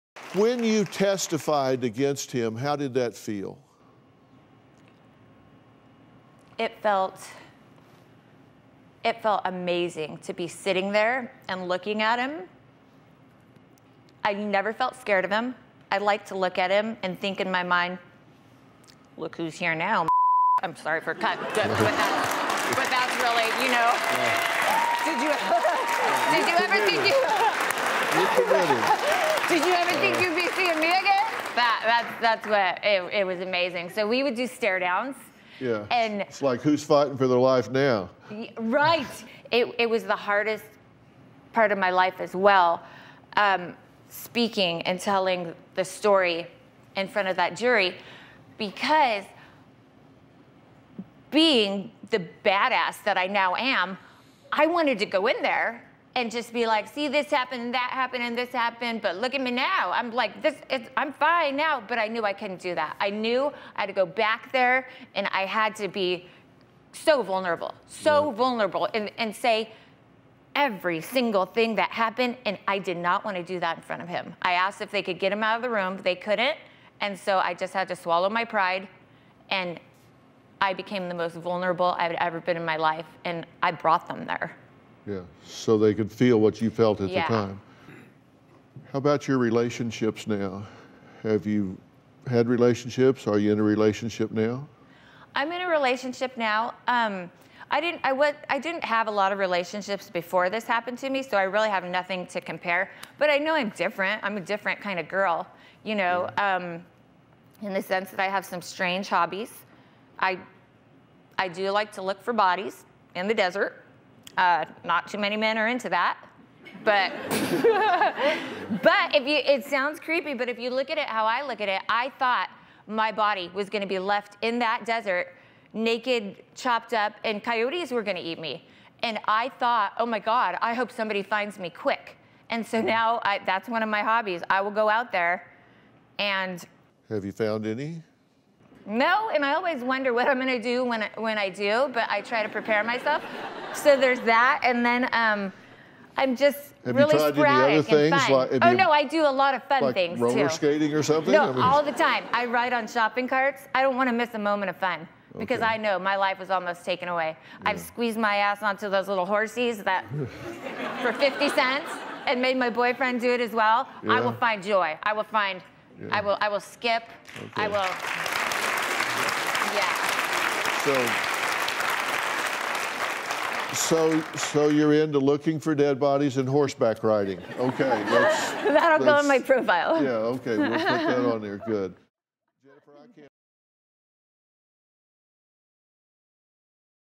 'We Would Do Stare Downs,' Says Woman About Testifying Against Serial Killer Who Kidnapped Her
A woman who escaped from a serial killer shares what it was like to face him in court.